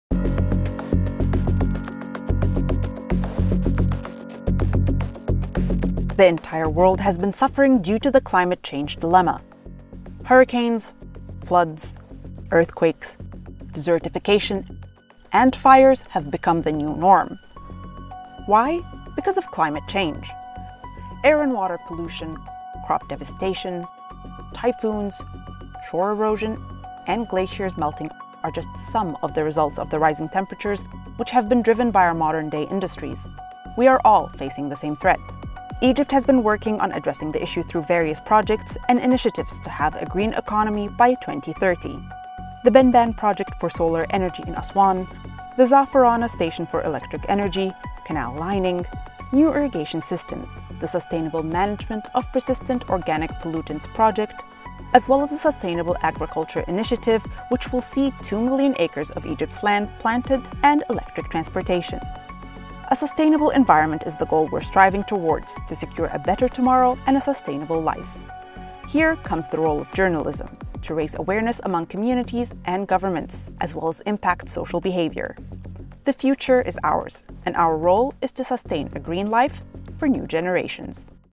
Climate Change Awareness Video
The climate crisis has become our new reality and only a shift to sustainability can save us.
Keep watching to learn more about Egypt’s contributions to combat global warming and the role
of journalism in securing a better, green future.